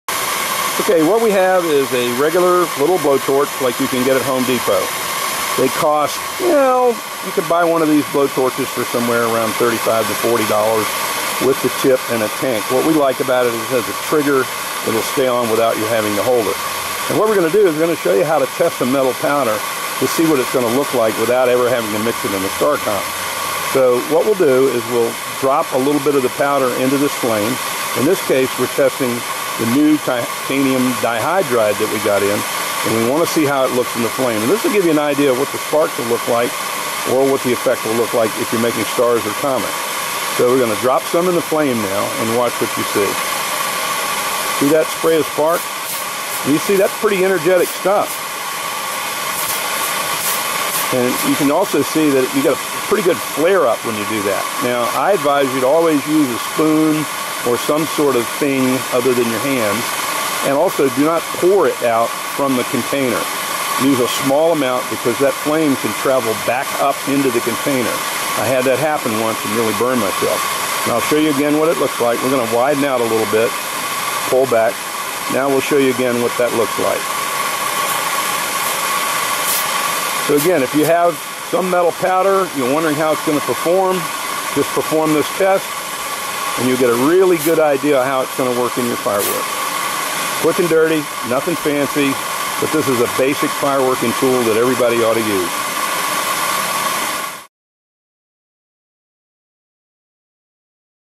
Testing Titanium for use in fireworks
Testing Titanium metal powder for fireworks This new Titanium Dihydride powder is only available at Skylighter.